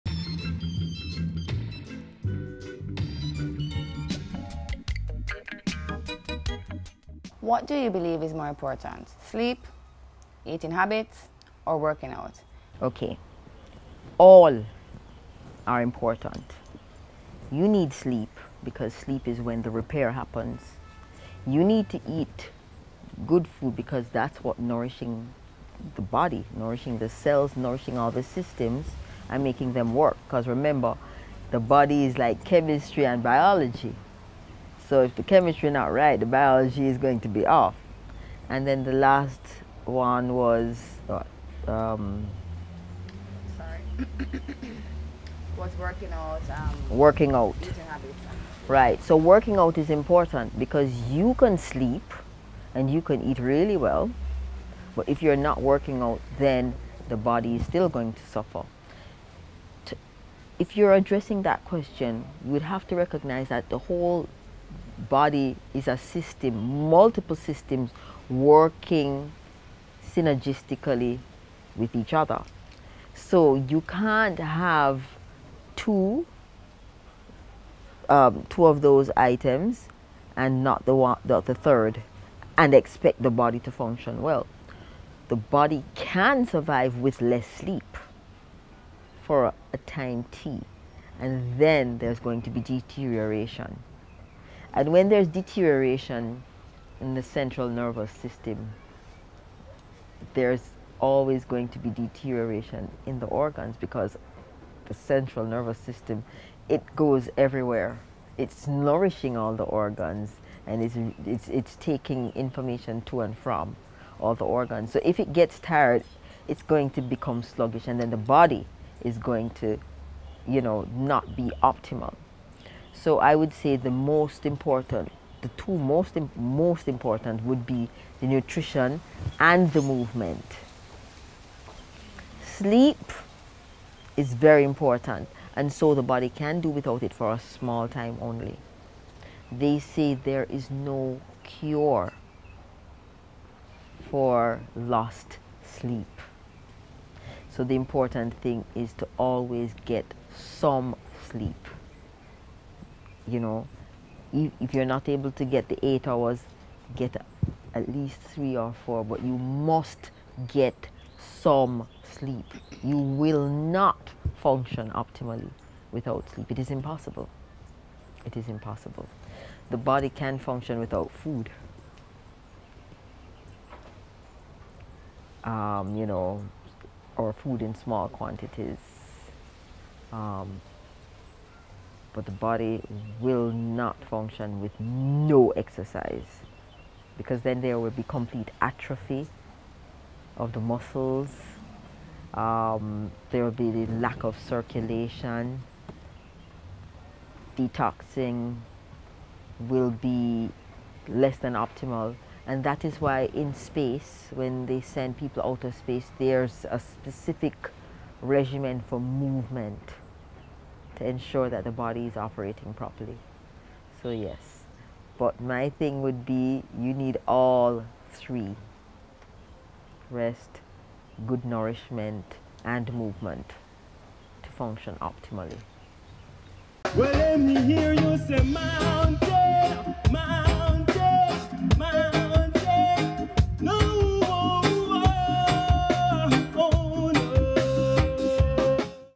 Paula Hurlock "If The Biology In Your Body Is Not Right, Your Chemistry Will Be Off" Pt.6
In this clip Paula Hurlock explains why sleep, diet, and exercise is of equal of importance when trying to maintain optimal health.